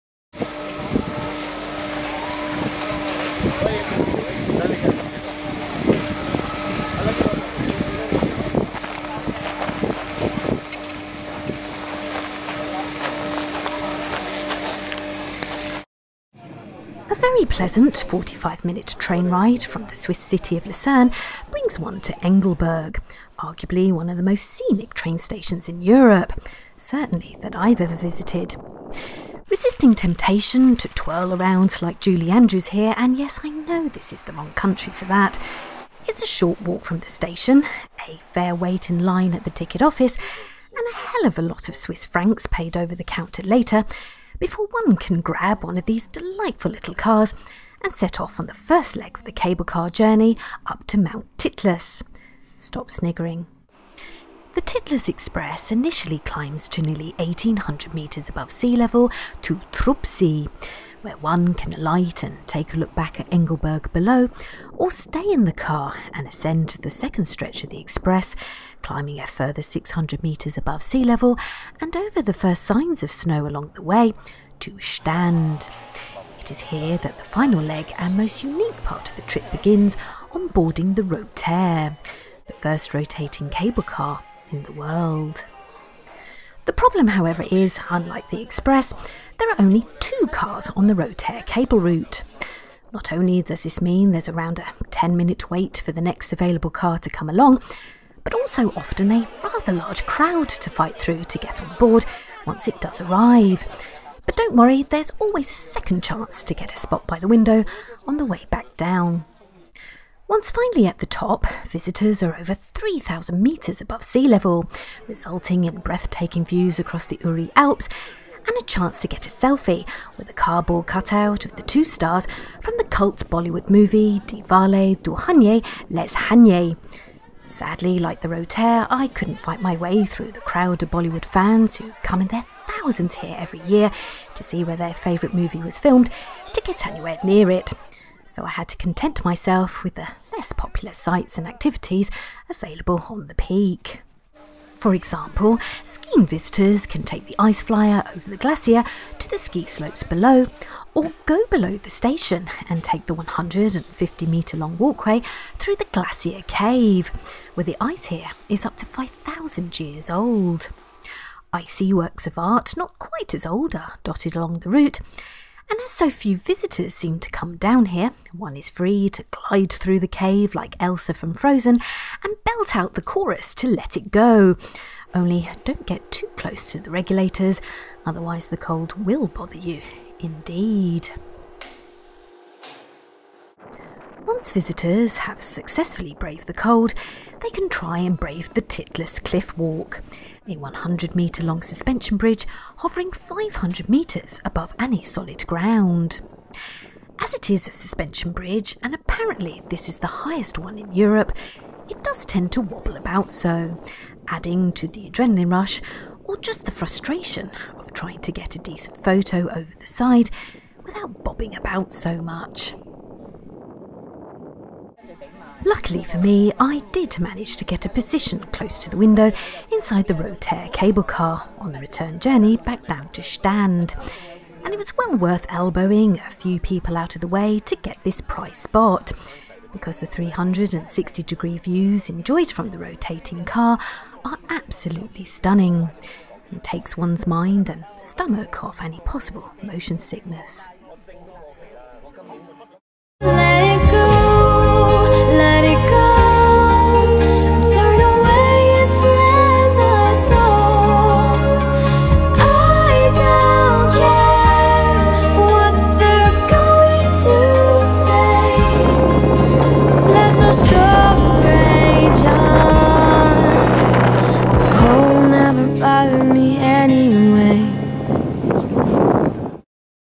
Mount Titlis, Switzerland: the Rotair cable car, the Ice Flyer and being Elsa from Frozen
One can reach Mount Titlis in Switzerland via the TITLIS Rotair: the world's first rotating cable car. Once at the top, visitors can have a selfie with a cardboard cut out of the stars from the Bollywood classic 'Dilwale Dulhania Le Jayenge', re-enact scenes from 'Frozen' inside the glacier cave, and walk across the TITLIS Cliff Walk, the highest and possibly wobbliest suspension bridge in Europe x